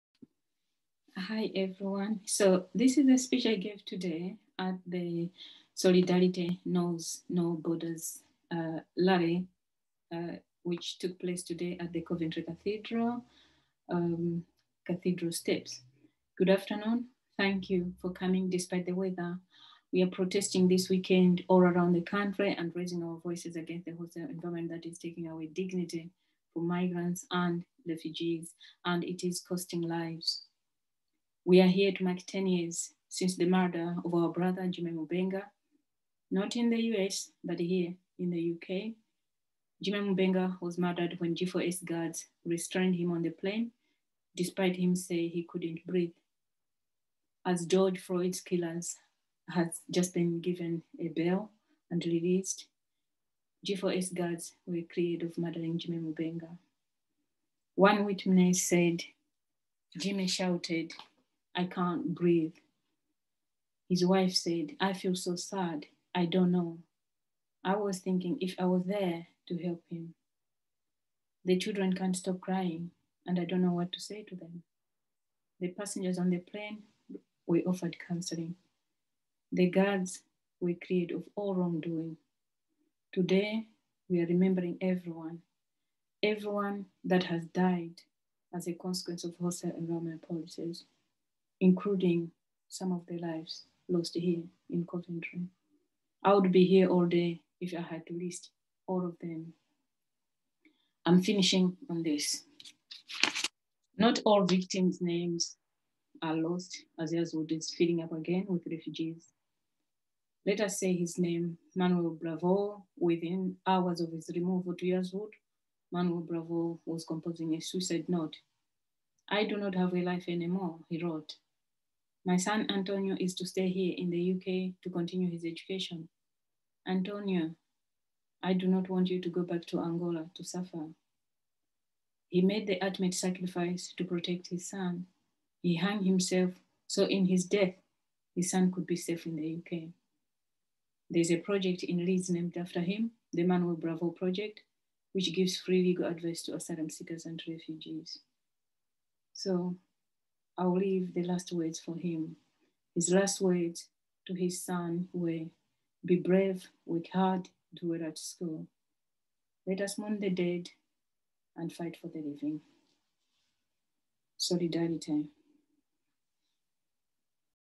My brother Jimmy Mubenga Speech
This weekend marks 10 years since the murder of our brother Jimmy Mubenga Speech